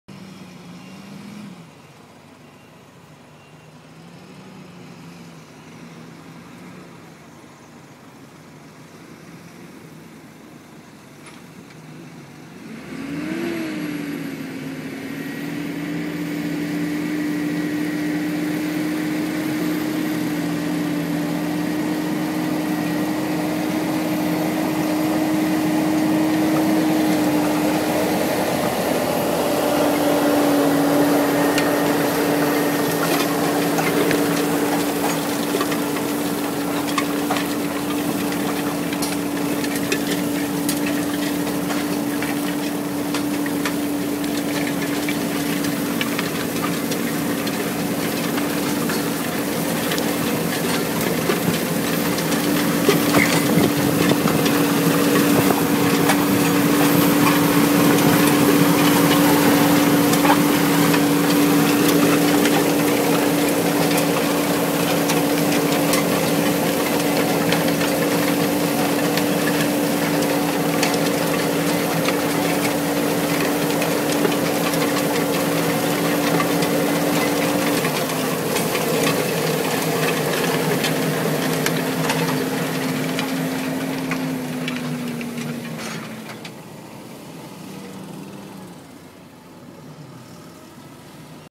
CSS Sumo Trio
Sowing Kale behind packer roller so discs can do more work